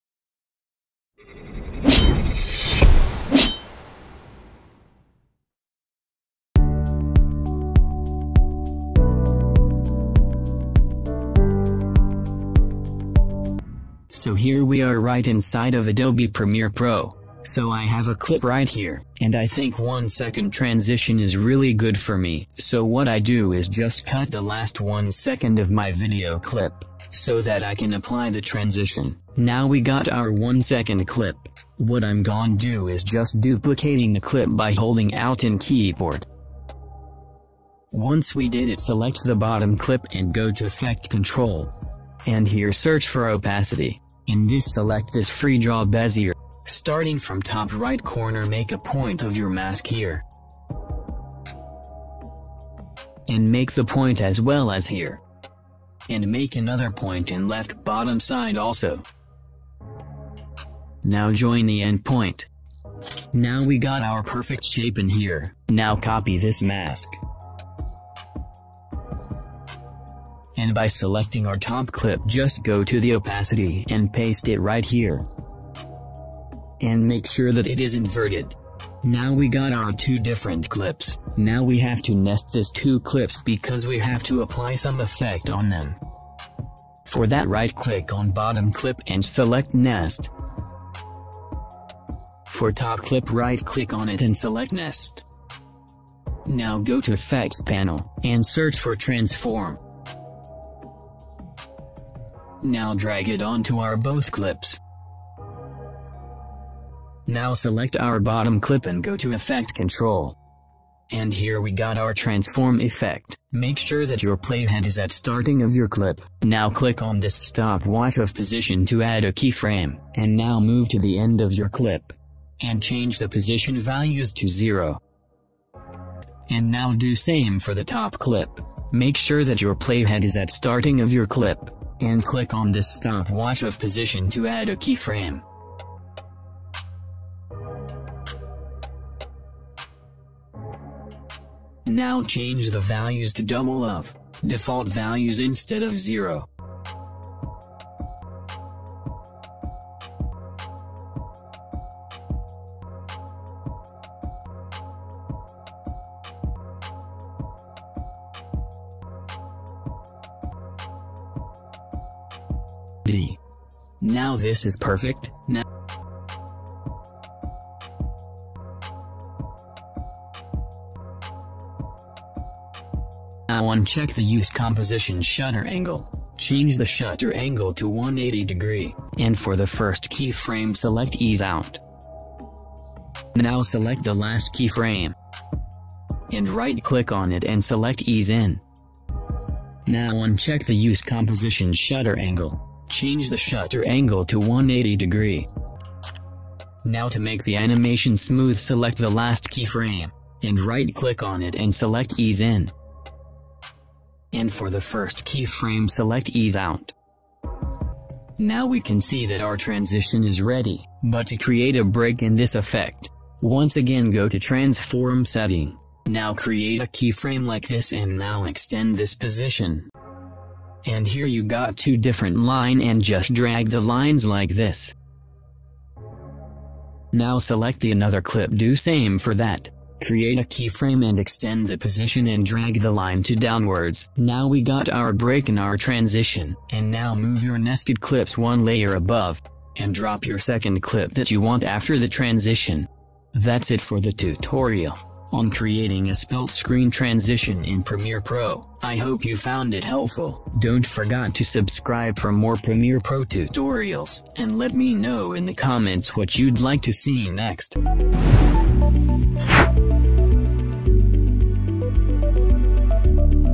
Split Screen Transitions in Premiere Pro | Step-by-Step Tutorial
In this video, I will show you how to create amazing split screen transitions in Premiere Pro. Whether you're a beginner or a pro, this tutorial will teach you everything you need to know to create seamless and dynamic split screen effects for your videos.

Using Premiere Pro's built-in tools and effects, we'll go through the process step-by-step, including setting up the split screens, adjusting the timing and motion, adding music and sound effects, and more. By the end of the video, you'll have the skills and knowledge to create your own split screen transitions that will take your videos to the next level.

Join me and let's master split screen transitions in Premiere Pro together!